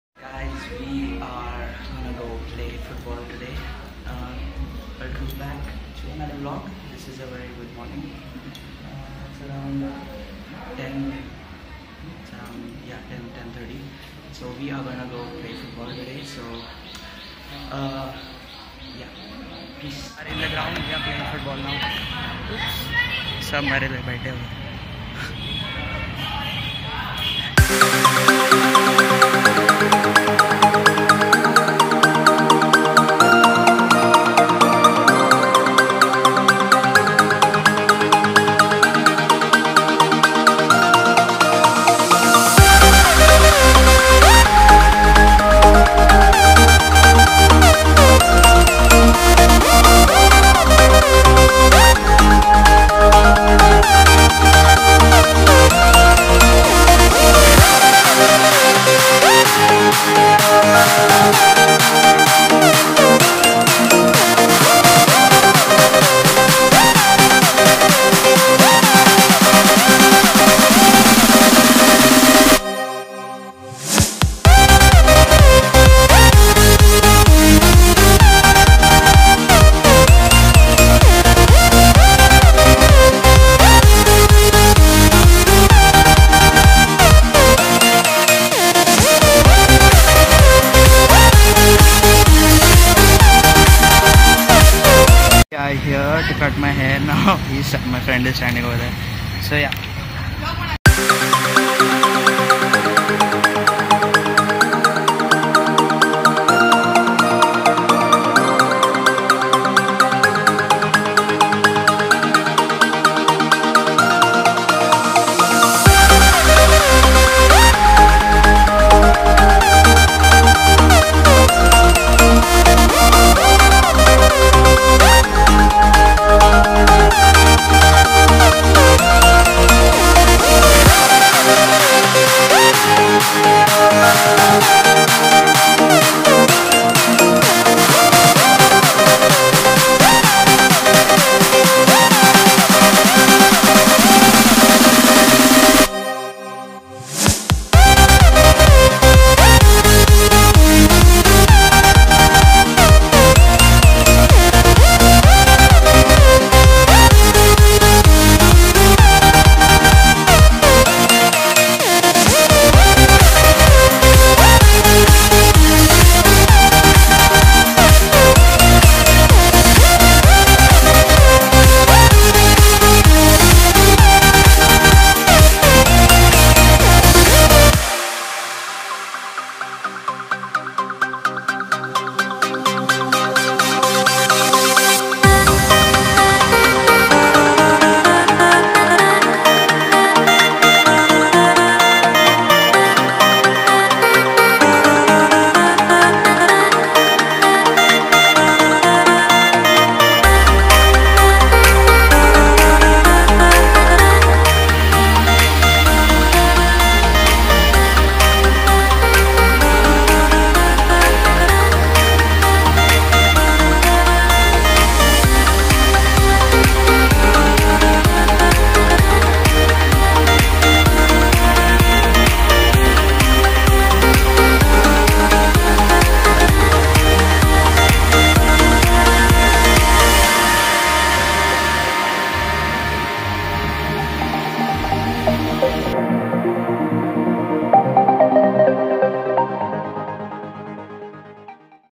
Day Of Football And Getting My Hair Cut Done.
This is my second vlog Hope you Guys Enjoyed The Video And Pls do Leave.